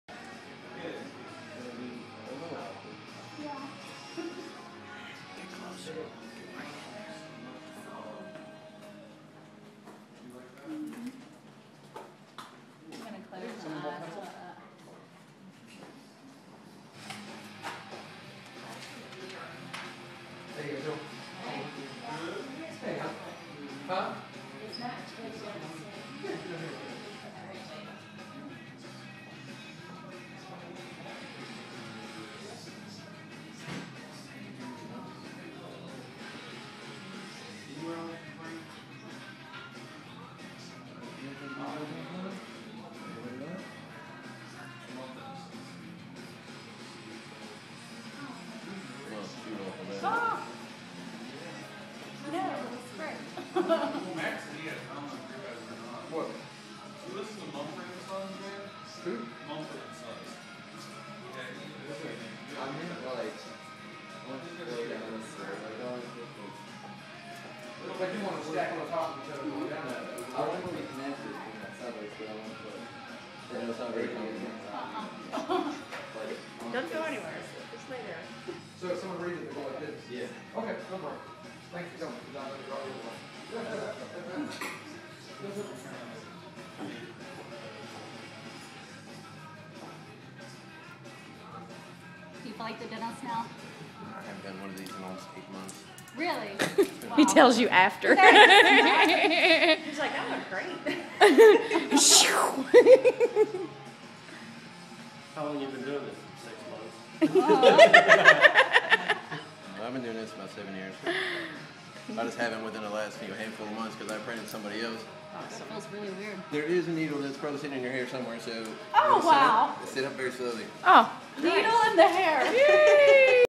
Amira's Monroe Piercing
Amira getting a Monroe piercing.